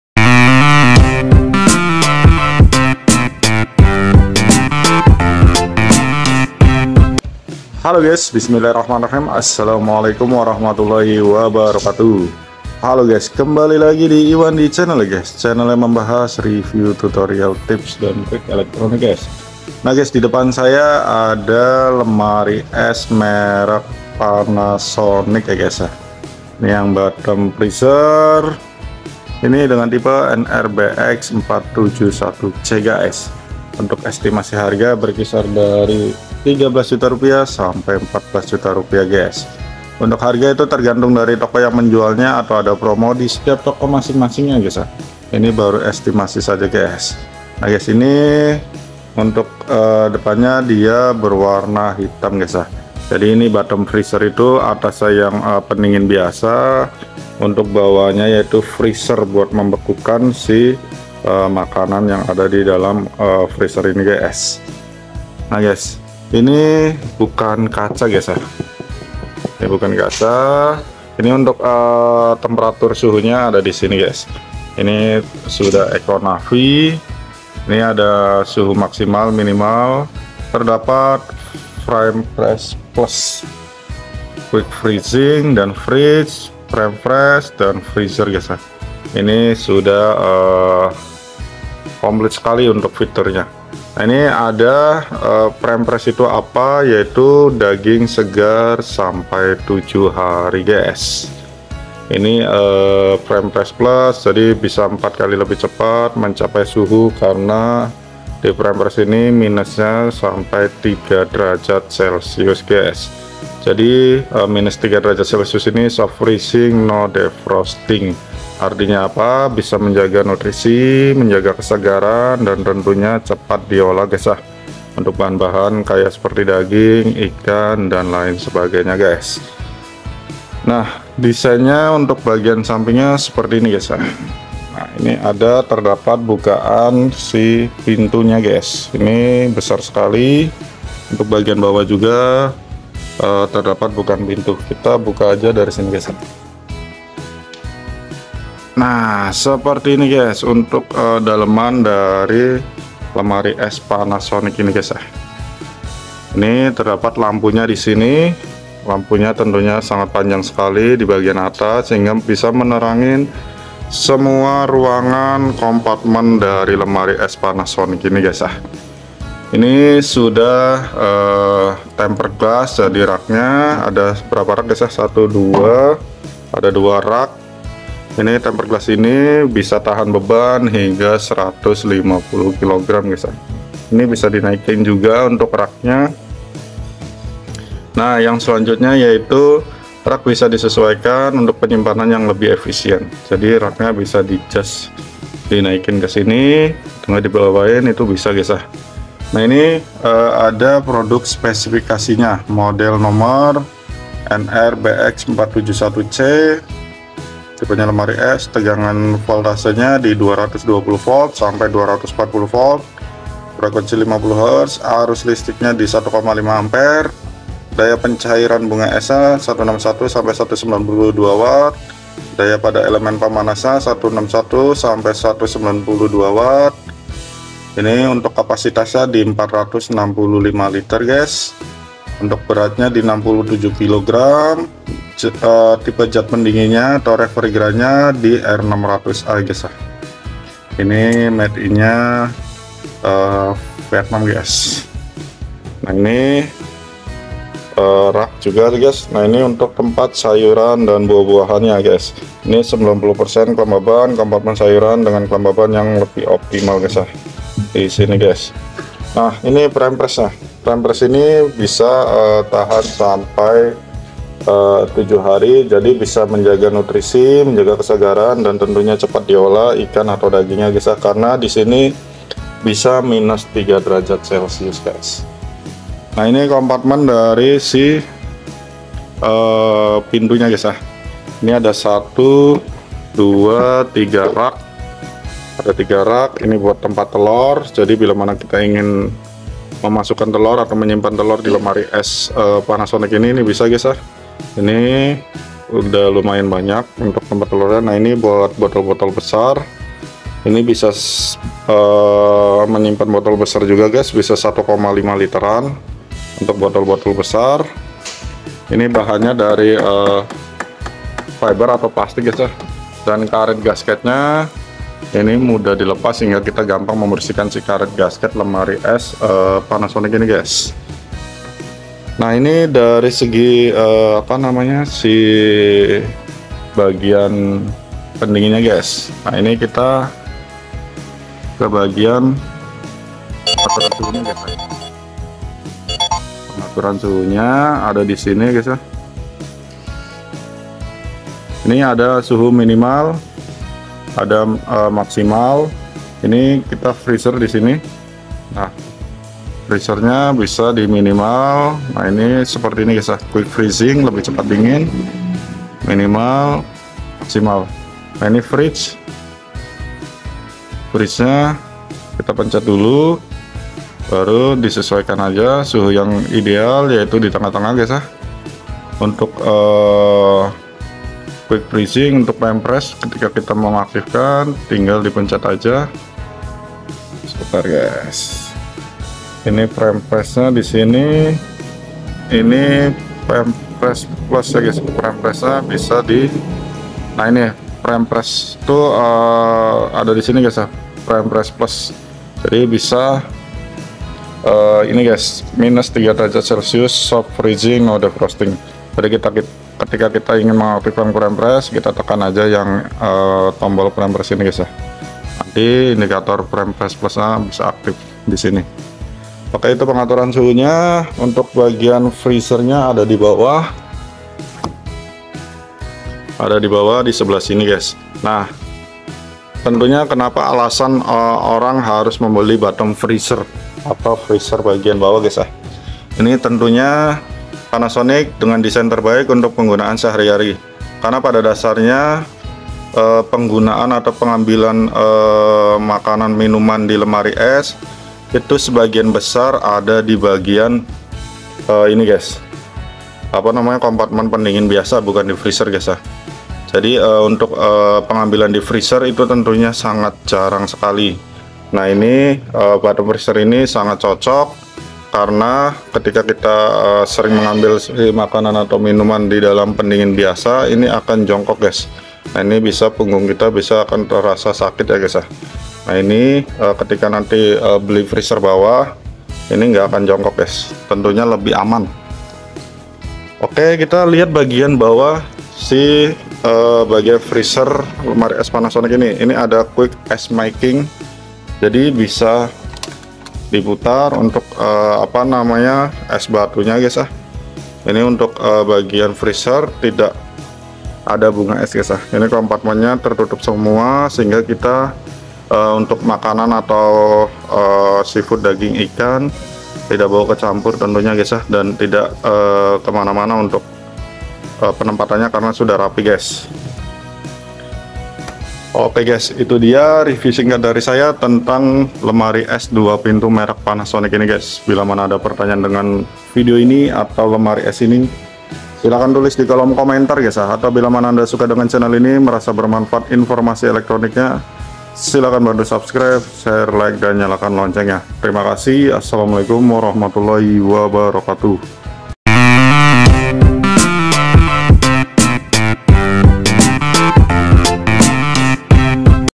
Kulkas 2 Pintu Panasonic 420 L Bottom Freezer NR-BX471C NRBX471C
Kulkas 2 Pintu Panasonic 420 Liter Prime Fresh Bottom Freezer NR-BX471C

Fitur :
- ECONAVI
- Prime Fresh+
- Ag Clean
- Compressor Inverter
- Bottom Freezer

Spesifikasi :
- Kapasitas Total 420 L
- Kapasitas Refrigerator 308 L
- Kapasitas Freezer112 L
- Product Dimensions (WxDxH) 686 x 695 x 1790 mm
- Door Color Glossy Silver Steel
- Compressor Inverter
- Refrigerant R600a